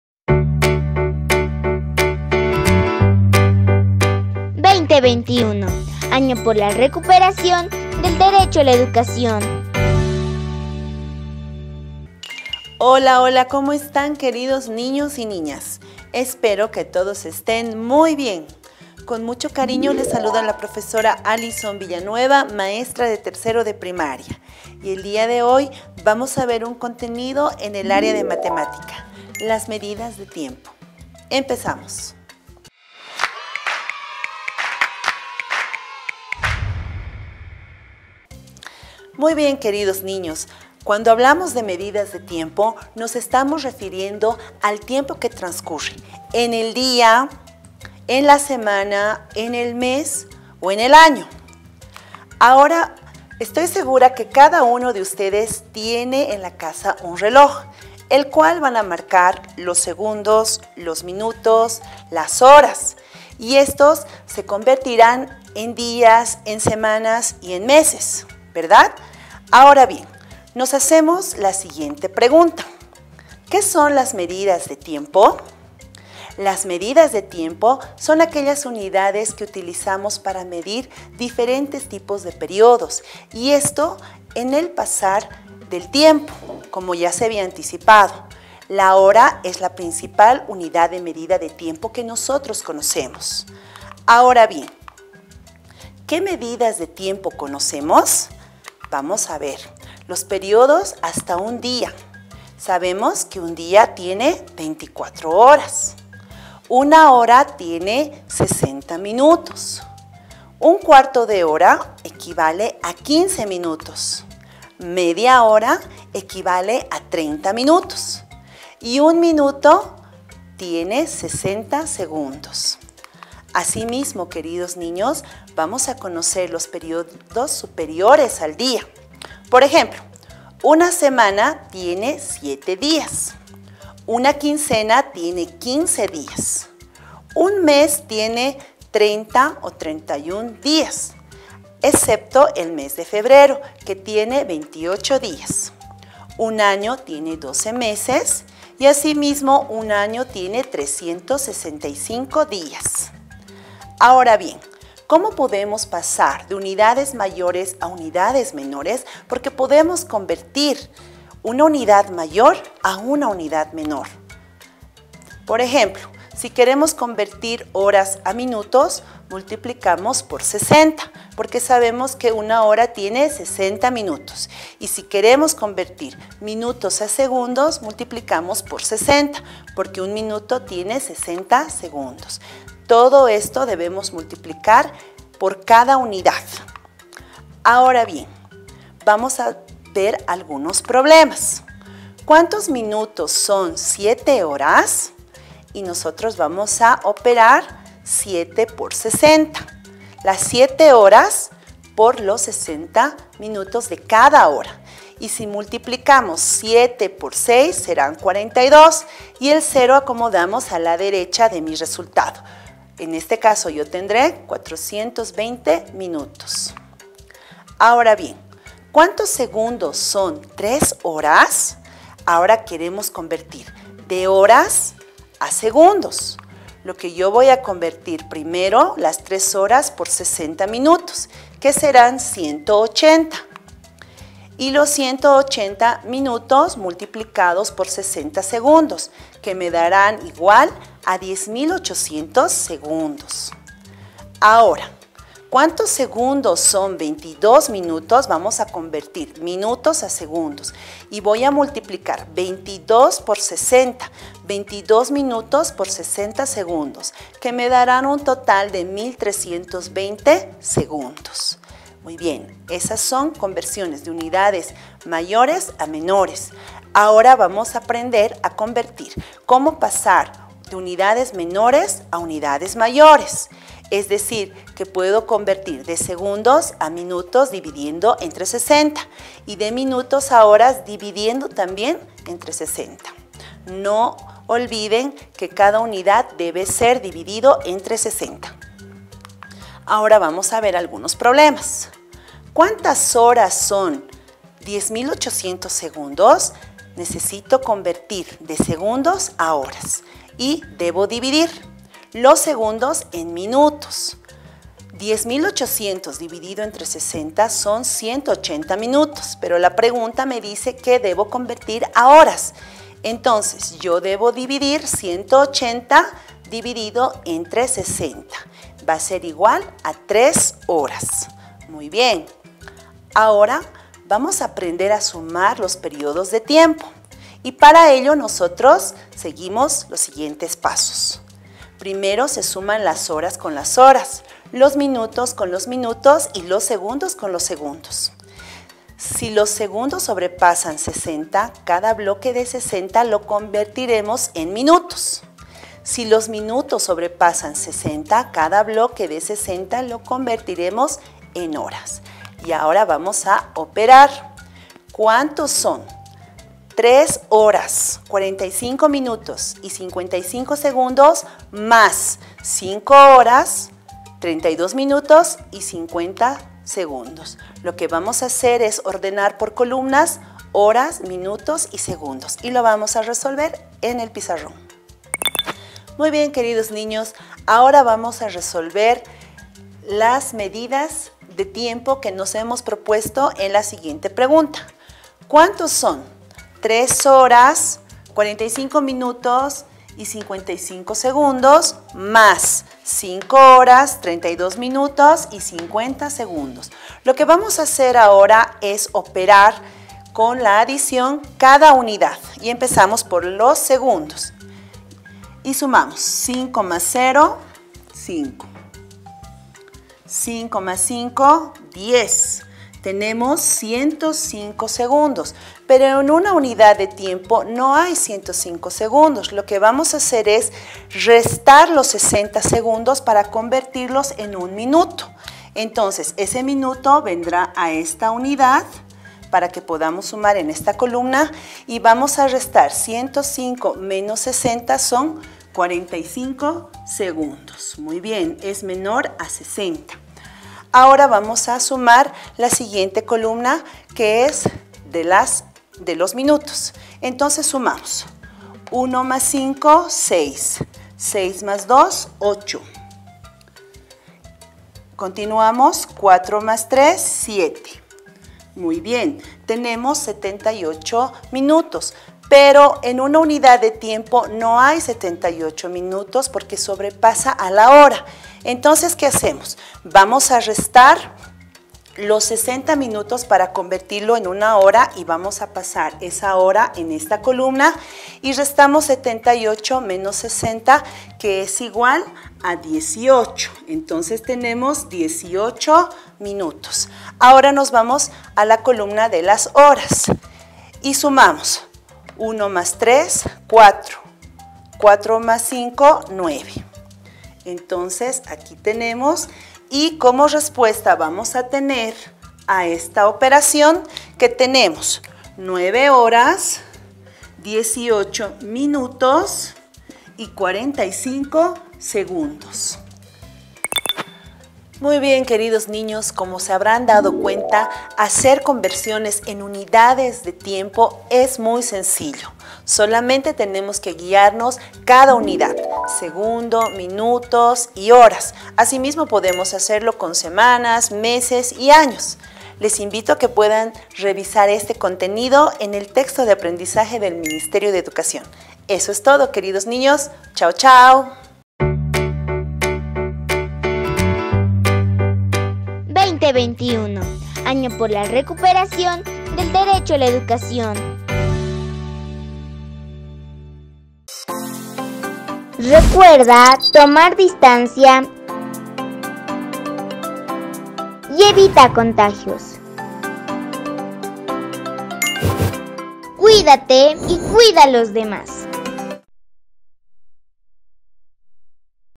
3 Primaria - Matemática - Medidas de tiempo
Nivel ✍️💻 👉 3er Año de EducaciónPrimaria Comunitaria Vocacional

Tema 👩🏫 👉 Matemática - “Medidas de tiempo”

Maestr@ 👩 👨 👉 Prof. Alizón Villanueva

Fecha 📅 👉 miércoles, 1 de diciembre de 2021